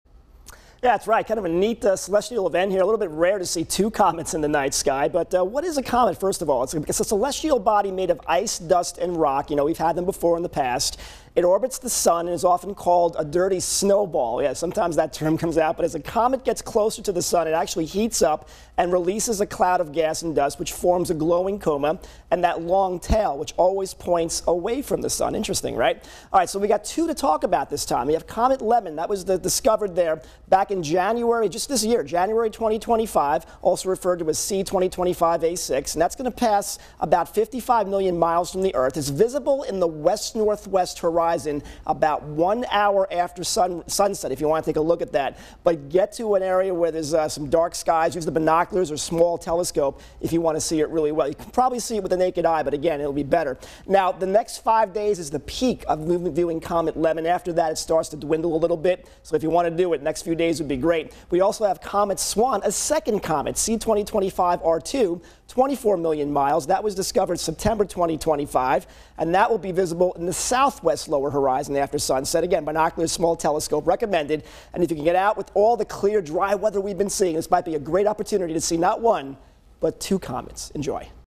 Comet Lemmon and Comet SWAN
Comet Lemmon and Comet SWAN will be visible in the night sky through early November.